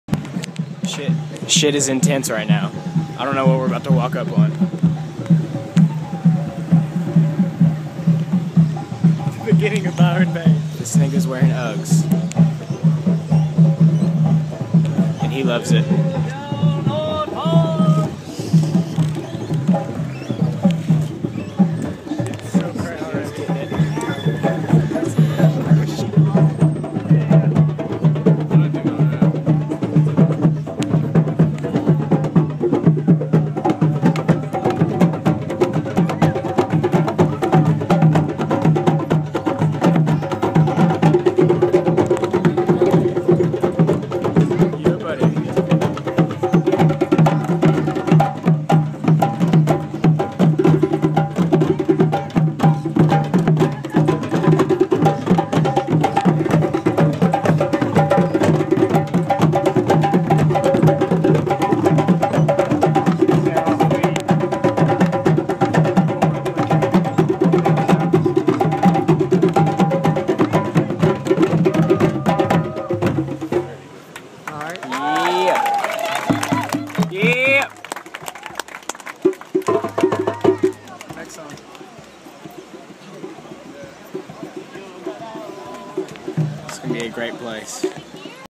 .OZ diaries/Crazy Byron drums
Byronians showing what this towns all about. The energy is electric as drums are played calling on the moonlight.